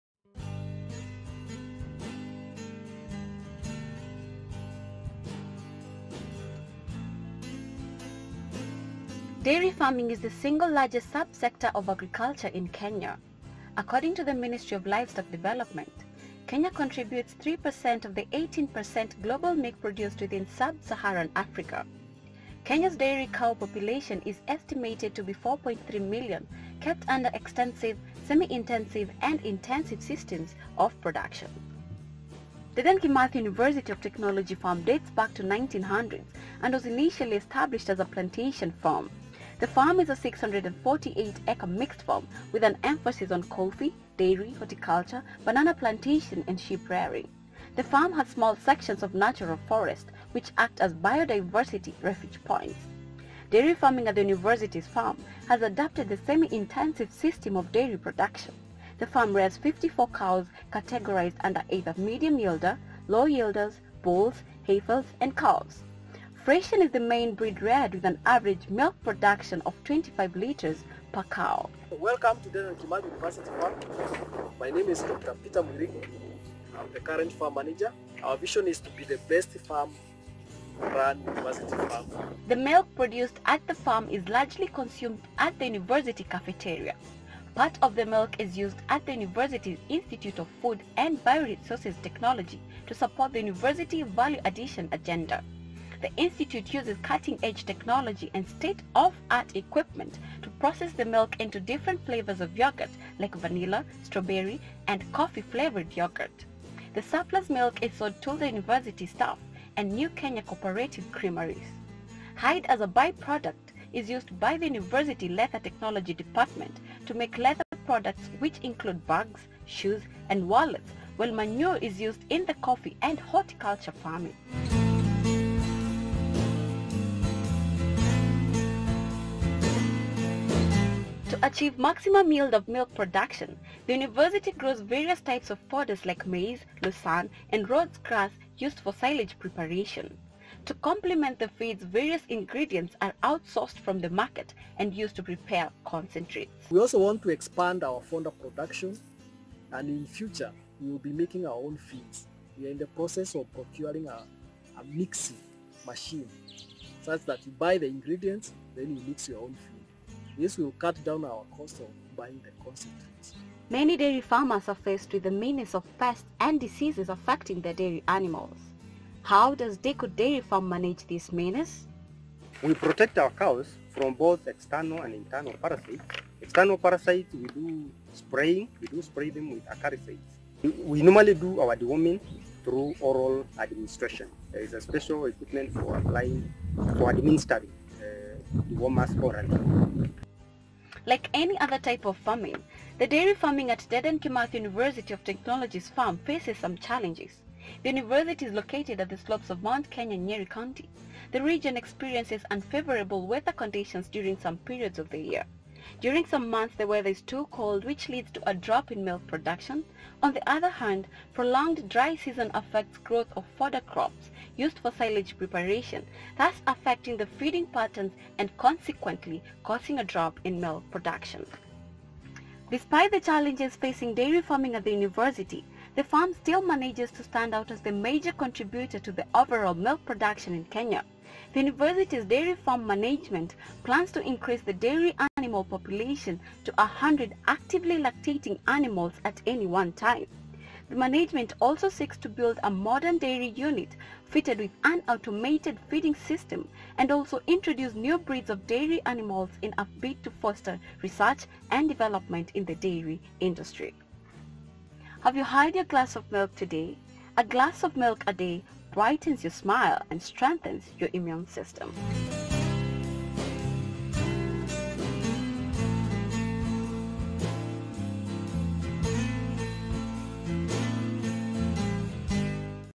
DeKUT Dairy Farming documentary
Learn more about Dairy Farming at dedan Kimathi University of Technology (DeKUT) Farm.